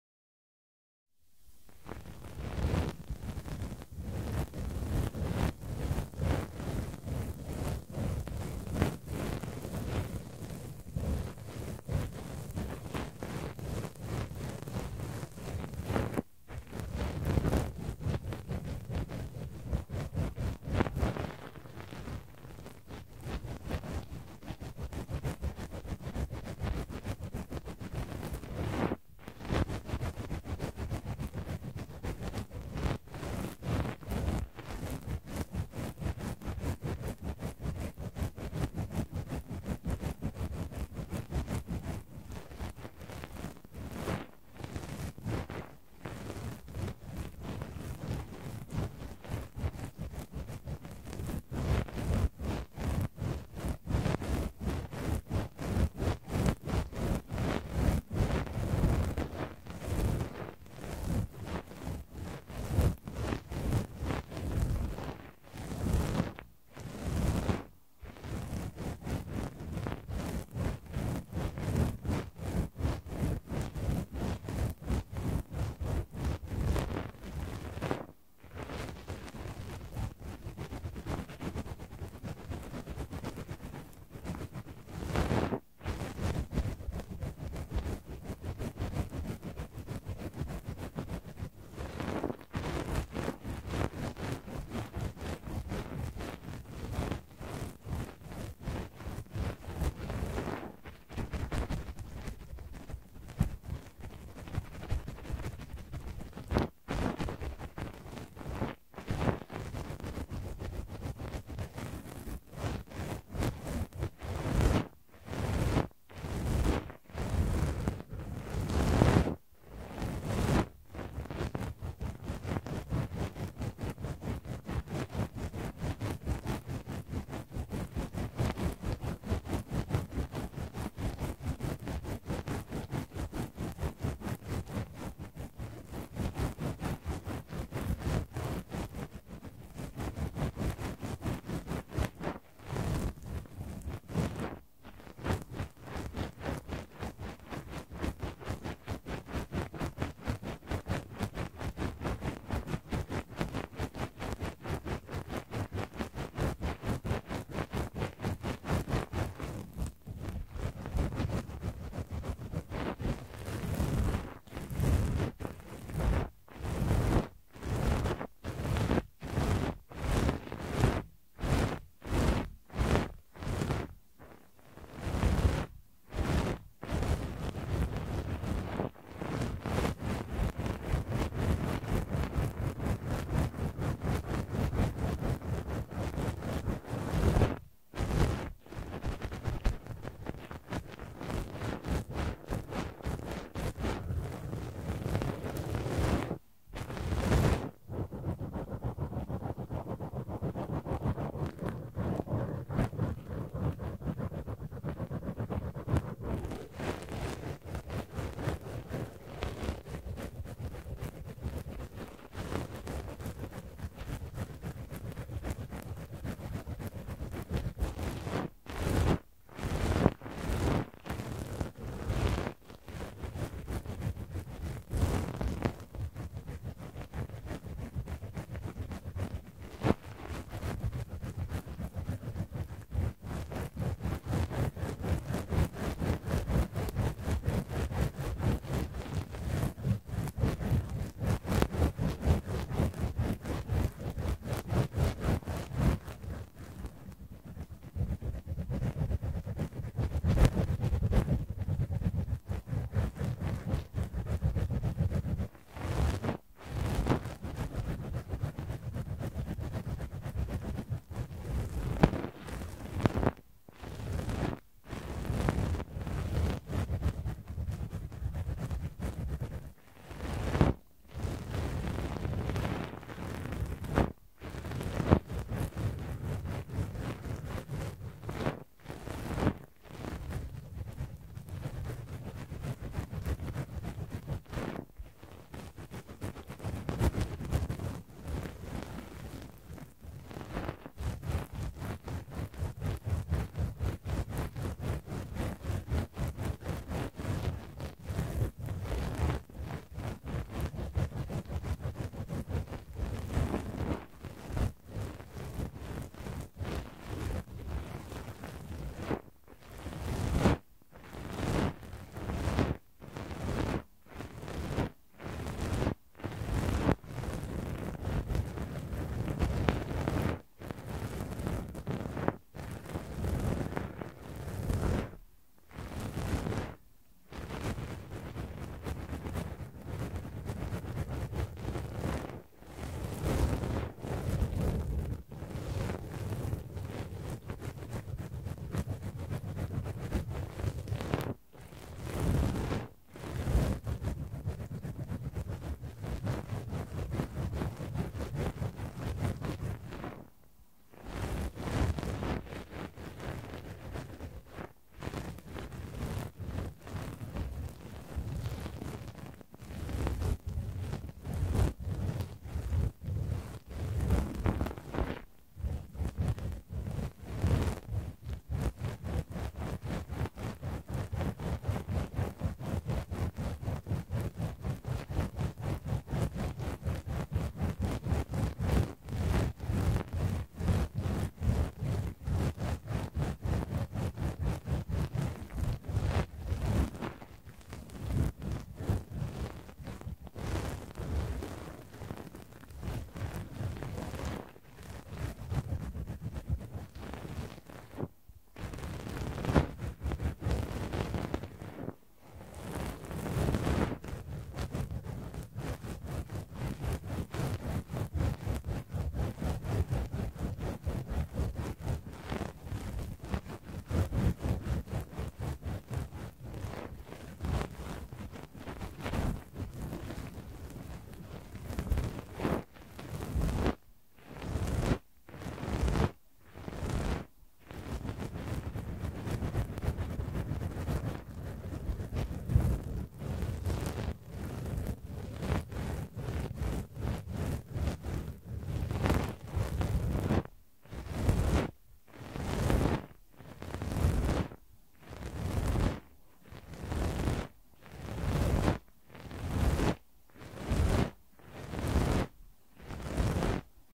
ゾワゾワしたい人集合。まじで / ASMR
アザラシのぬいぐるみです！イヤホンを付けてお楽しみください。